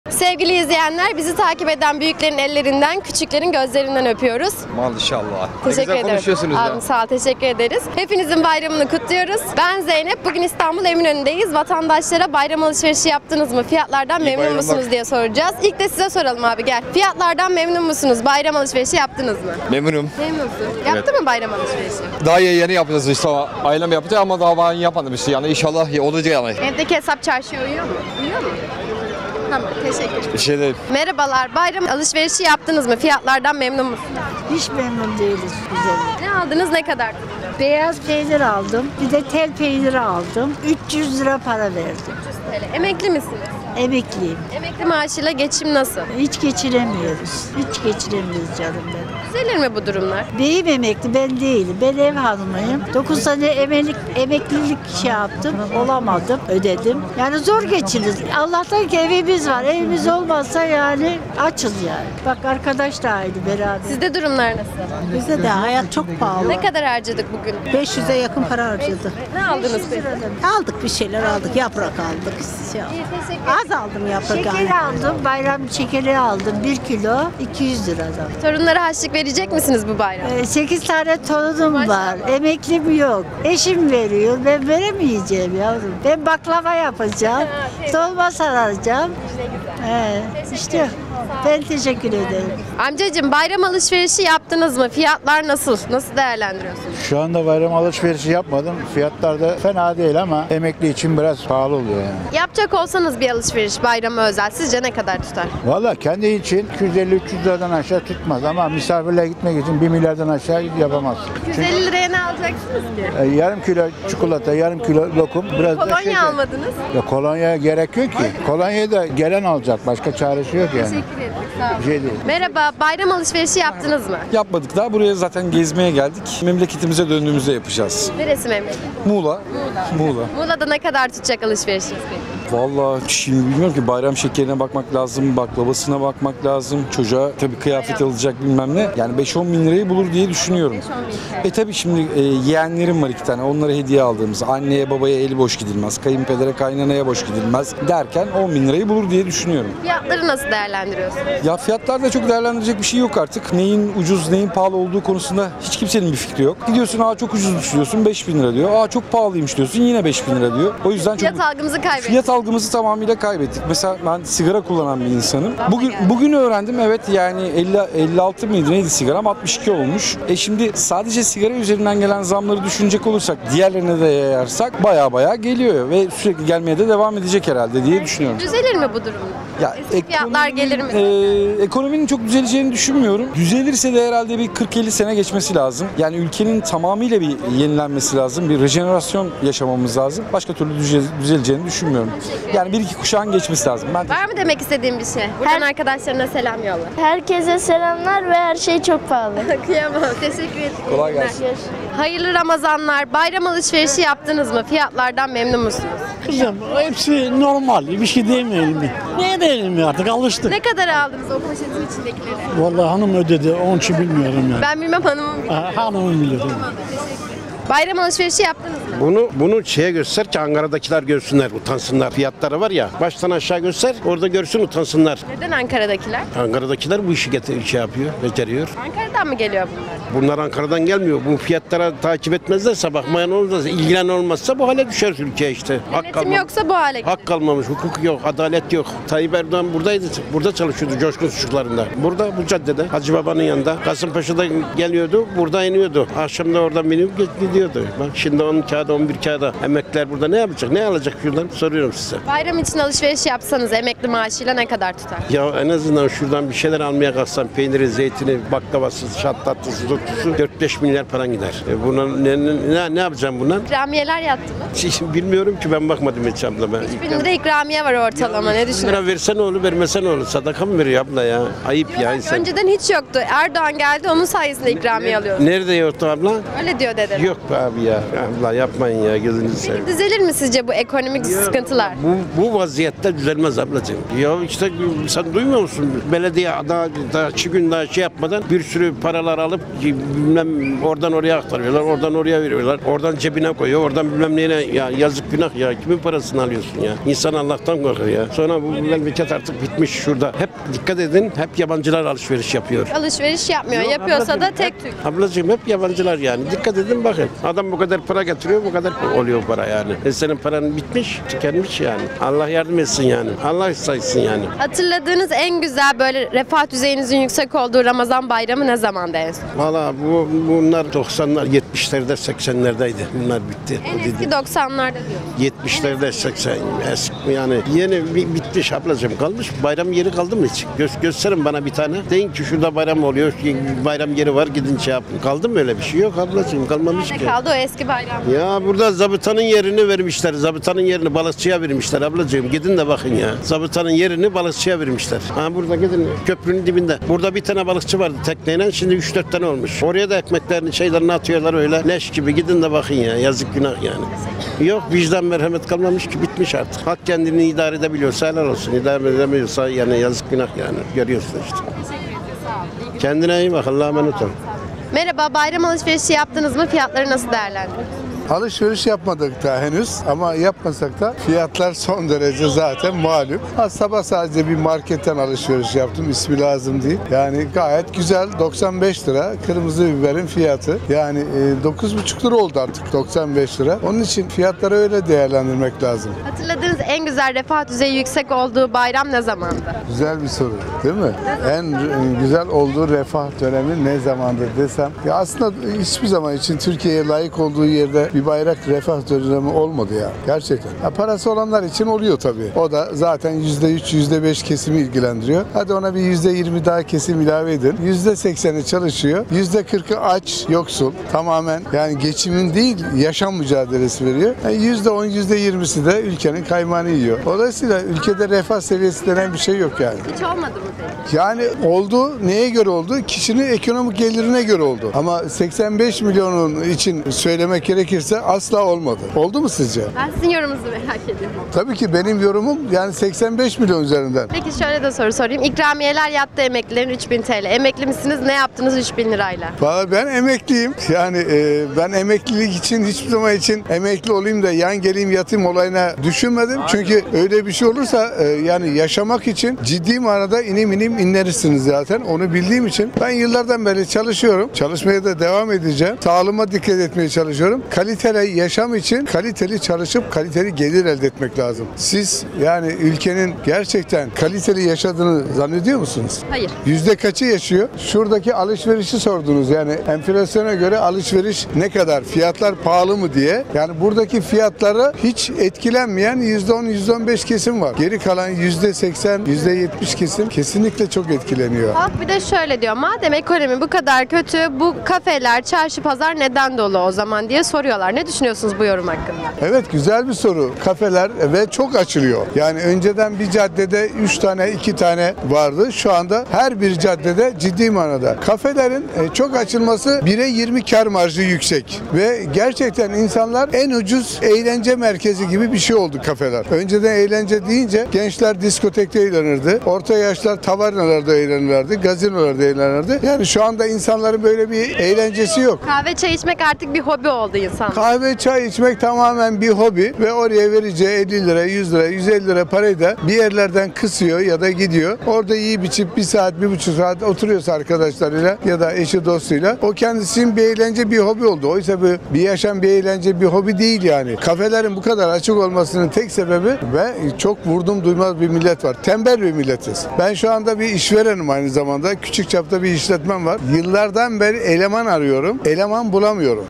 BAYRAM ALIŞVERİŞİ YAPTINIZ MI? "FİYAT ALGIMIZI KAYBETTİK..!" | Ekonomi | Gündem | Sokak Röportajları
Sokak röportajları ile halkın nabzını sokakta tutmaya hız kesmeden devam ediyoruz...